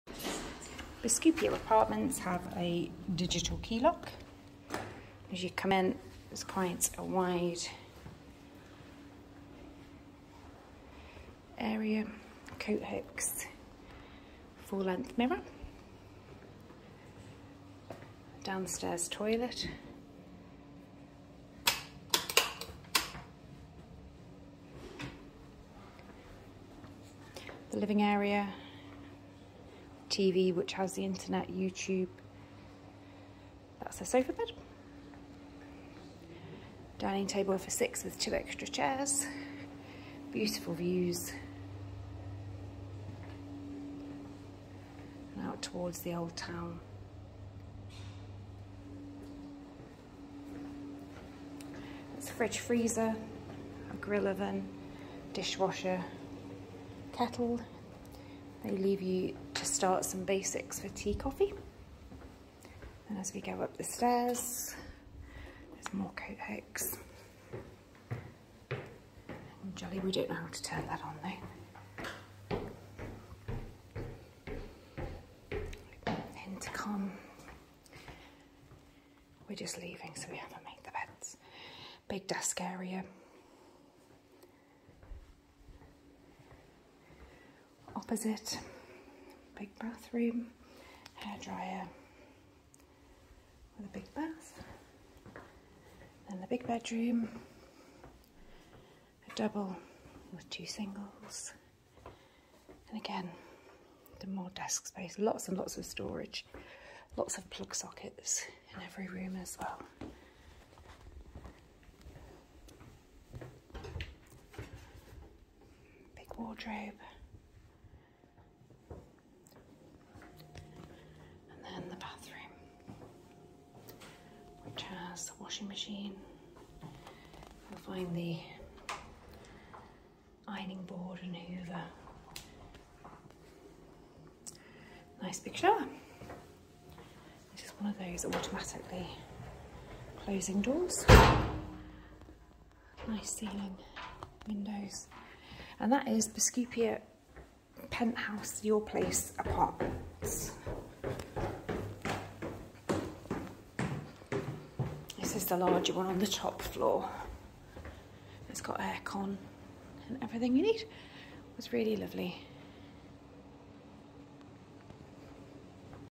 Yourplace Biskupia apartment pent house to sleep 8 Krakow Poland
quick walk round tour of pent House apartment. Krakow Poland
Yourplace biskupia